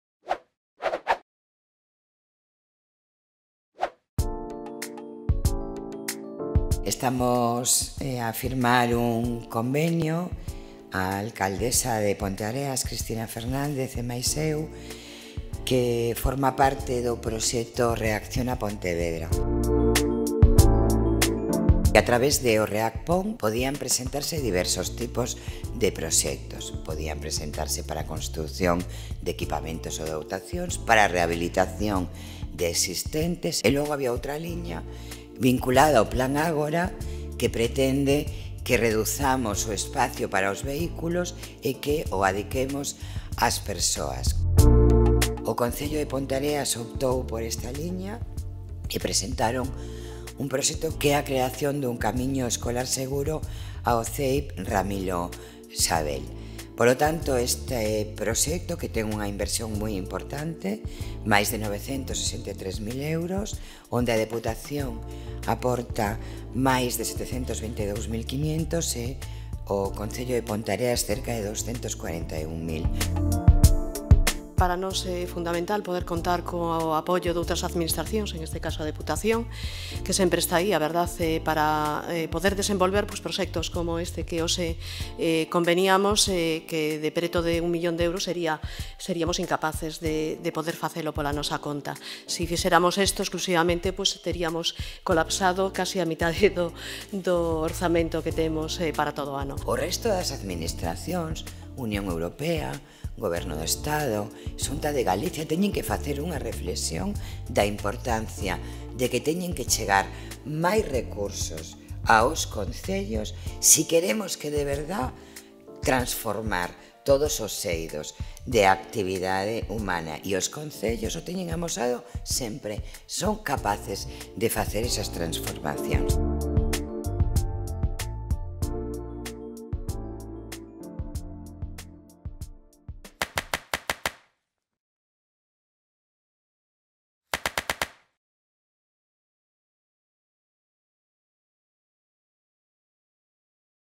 Plan ReacPon - Deputación e concello de Ponteareas crean un camiño seguro ao CEIP Ramiro Sabell
A presidenta da Deputación, Carmela Silva, e a alcaldesa de Ponteareas, Cristina Fernández, asinan o convenio do Plan ReacPon co que o concello do Condado inicia a construcción dun camiño escolar seguro ao CEIP Ramiro Sabell, unha intervención que conta cun presuposto de 963.527,36€.